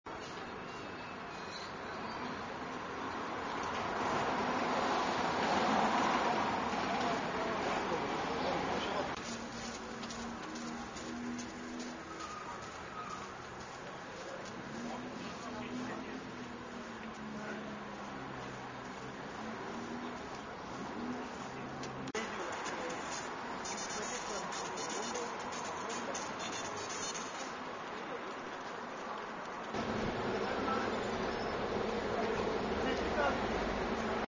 Izmir Aliağa'daki patlama
İzmir Aliağa ilçesindeki Emniyet Müdürlüğü binasında meydana gelen patlamada, B.K. isimli bir polis memurunun iki elinin de koptuğu öğrenildi. Ege Üniversitesi Tıp Fakültesi Hastanesi'ne kaldırılan polisin durumunun ağır olduğu bildirildi.